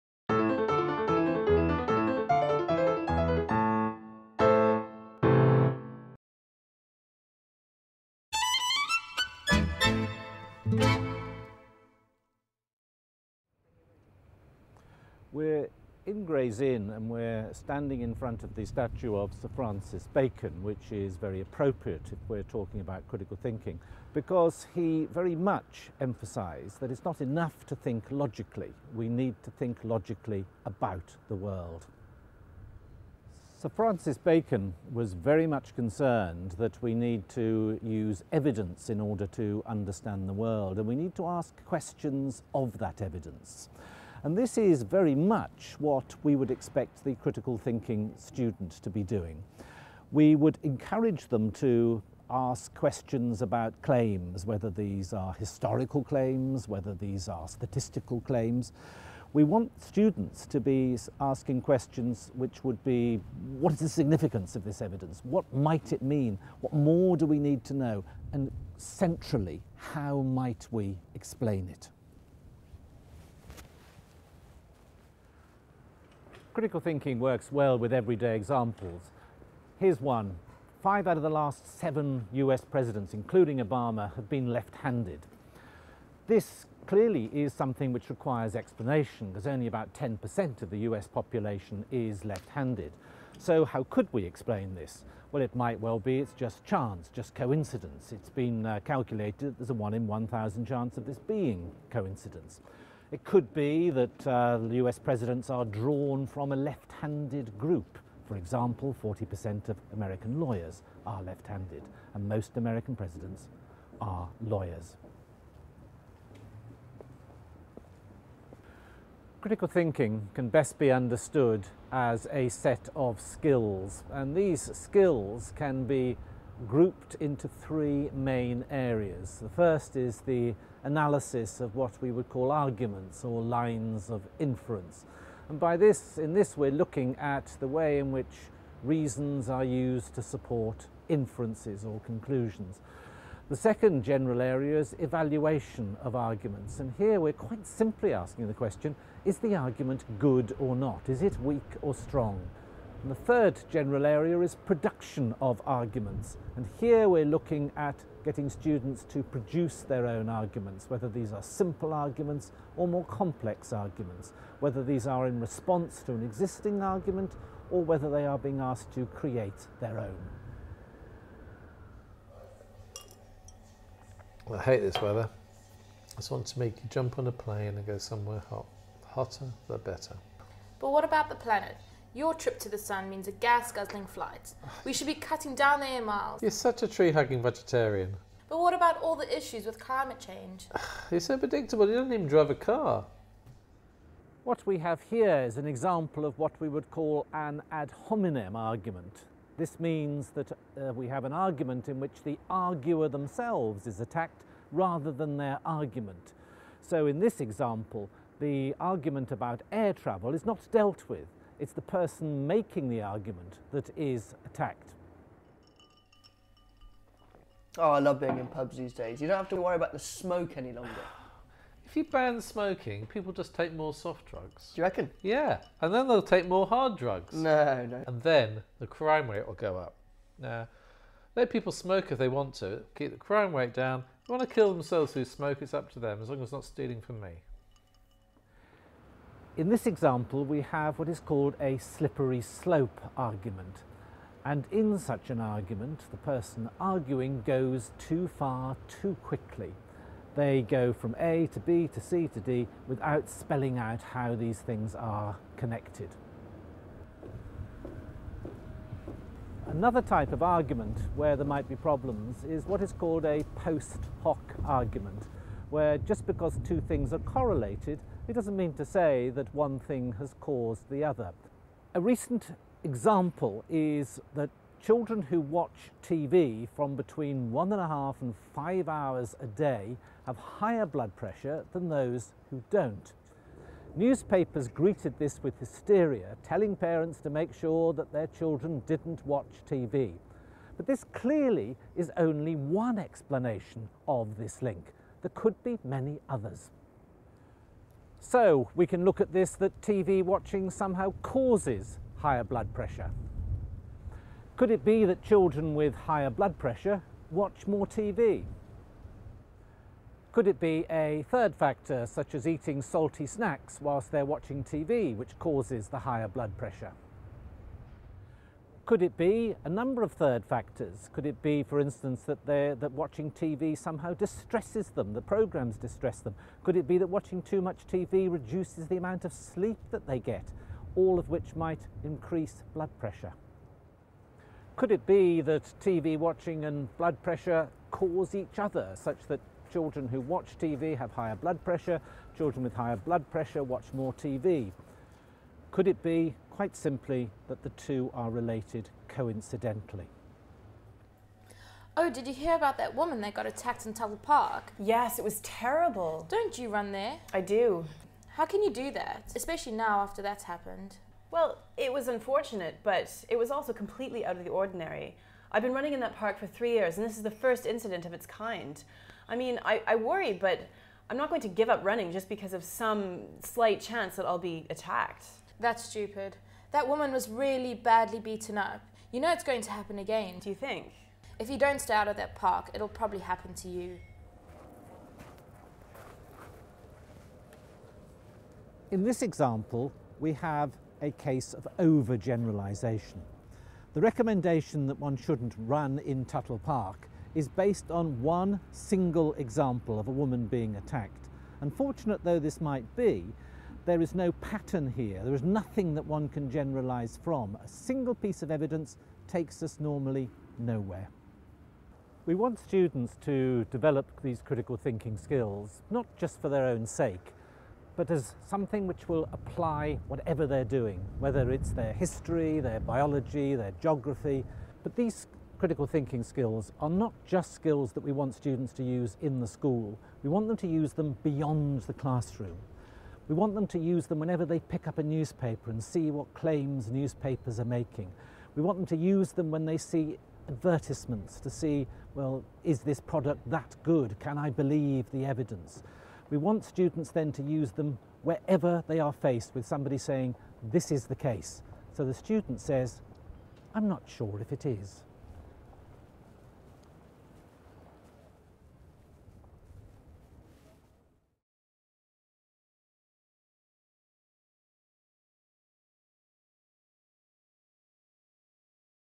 Critical Thinking
Roy van den Brink-Budgen, a former chief examiner for critical thinking, explains some of the concepts involved in helping your pupils developing critical thinking skills.
Learning how to analyse, evaluate and produce arguments can help pupils become enquiring thinkers, and skills can be developed across subjects and key stages.
Whatever the subject, critical thinking encourages students to take claims and ask questions about them, whether they are made in conversations, in the classroom, or in the media.